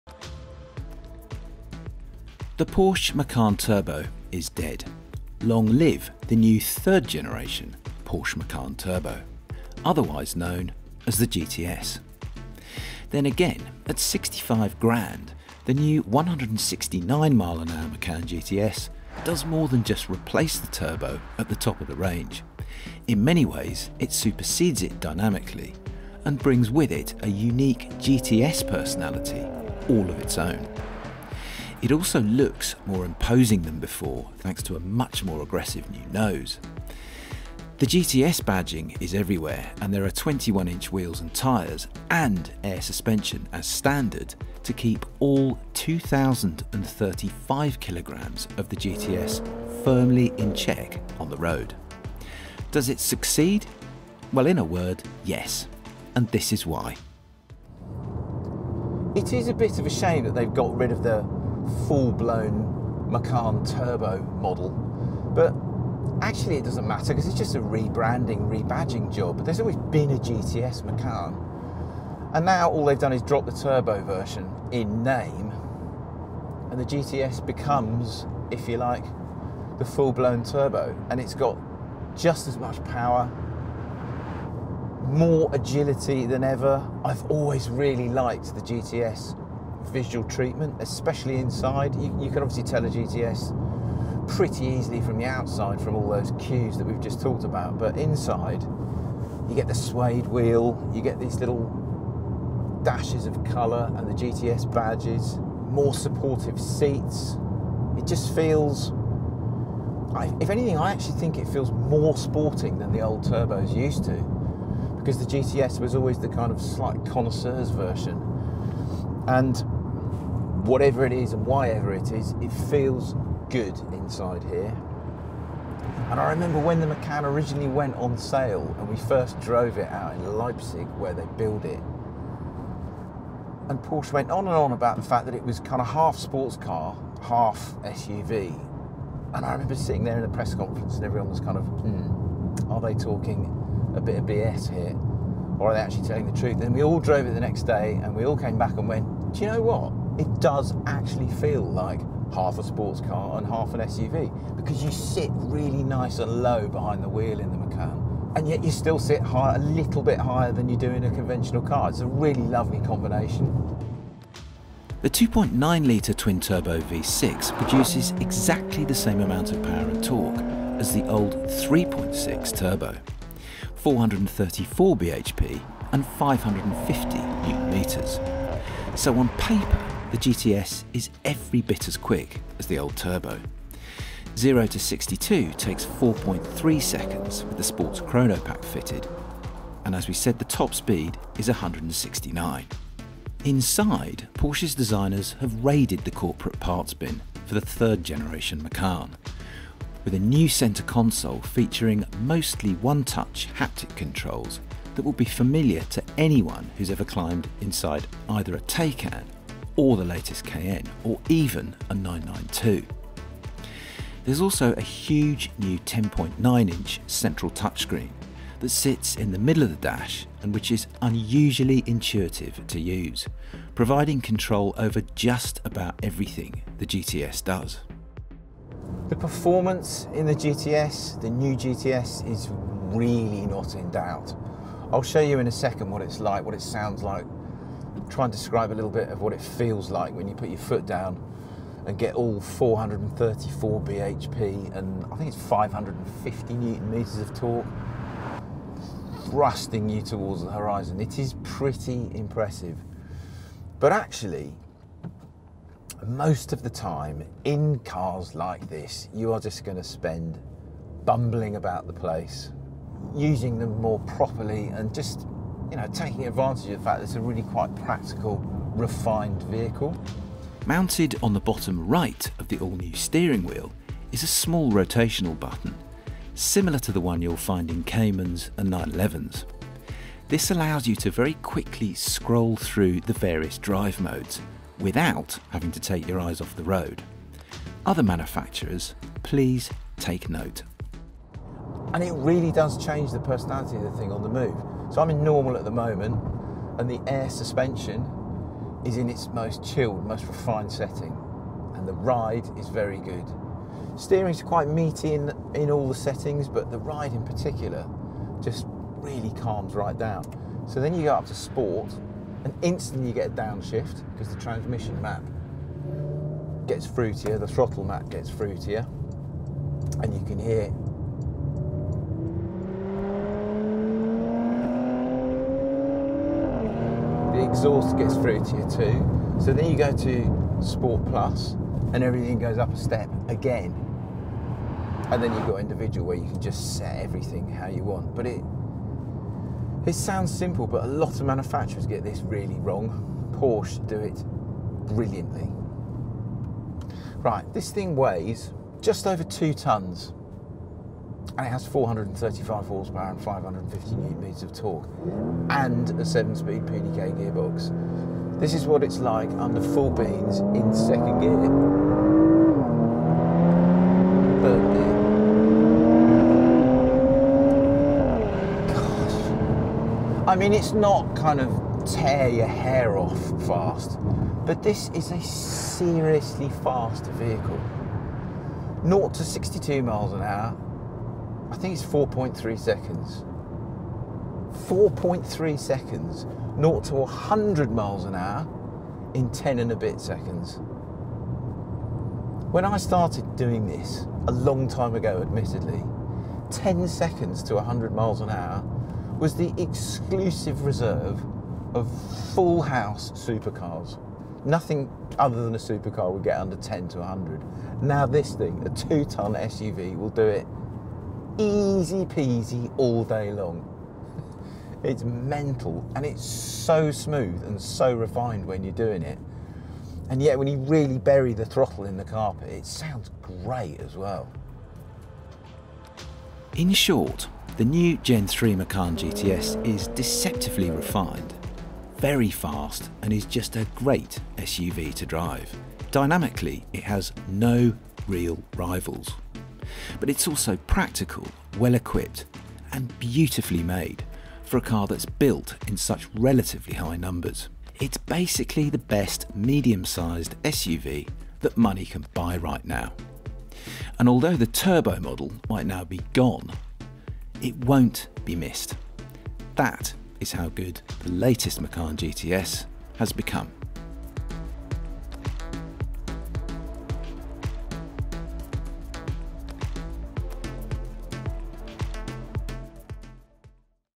NEW Porsche Macan GTS review: Steve Sutcliffe drives the fastest Macan on sale | Auto Express
Steve Sutcliffe drives the new, 2021 Porsche Macan GTS - is this the best hot SUV you can buy?

Porsche has revamped the Macan for 2021, and we put Steve Sutcliffe behind the wheel of the all-new, range-topping GTS model to find out if the hot SUV can still mix it with the best. The new GTS replaces the Macan Turbo as the hottest variant of Porsche's baby SUV, and packs the firepower to warrant the badge. A 2.9-litre twin-turbocharged V6 motor sends 434bhp and 550Nm of torque to all four wheels, bringing the car from 0-62mph in just 4.3 seconds. Our man Sutcliffe puts the facelifted model to the test.